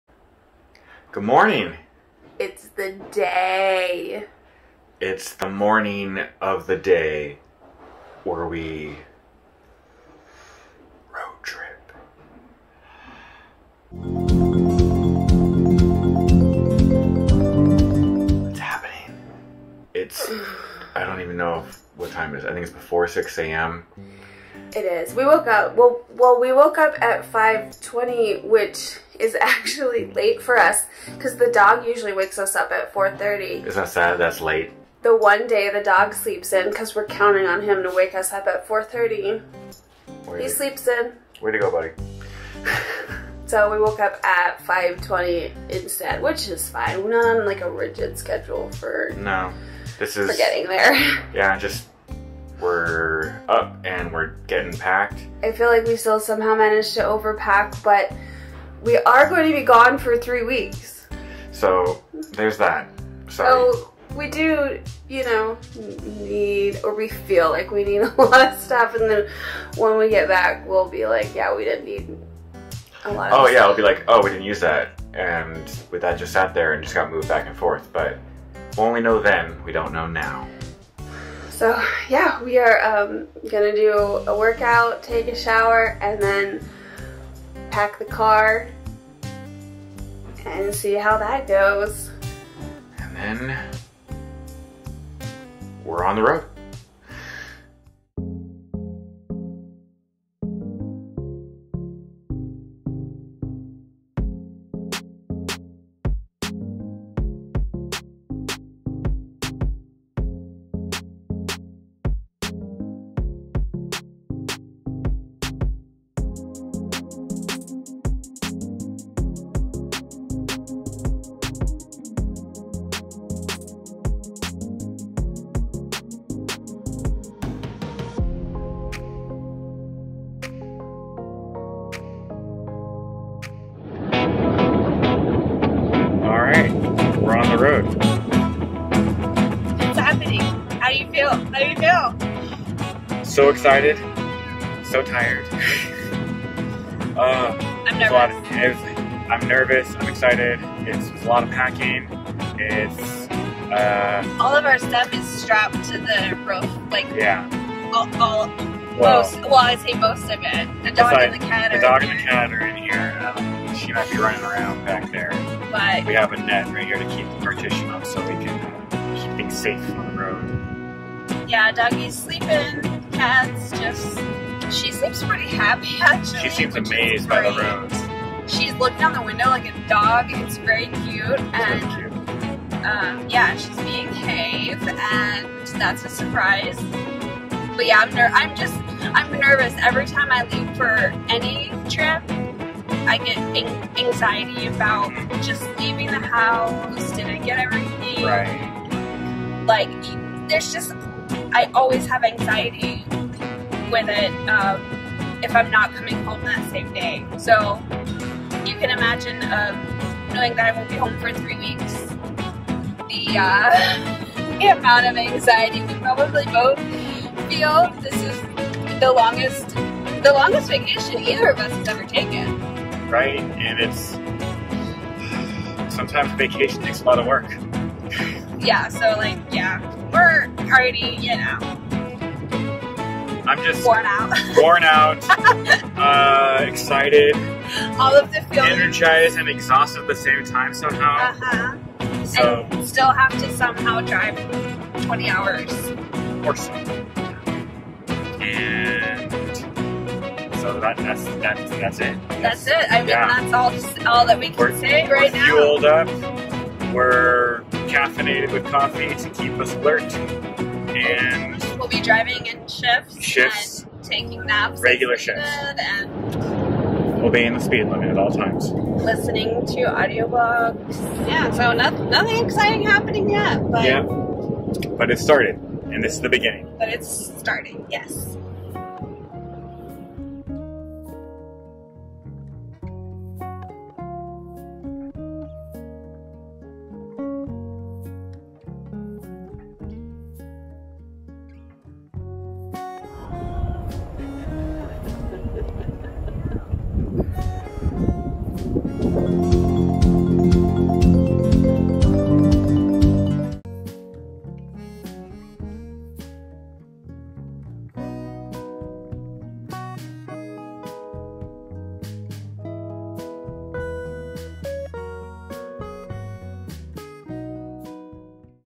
2020 24 hours to Texas Day 1 of our cross country road trip to Disney World
Emily and Alex from WeveGotEars hit the road. See us pack up the car and travel from California to Texas as the 1st leg of our cross country road trip to Disney World!

Rooftop Cargo Carrier Bag

Pet Barrier for Car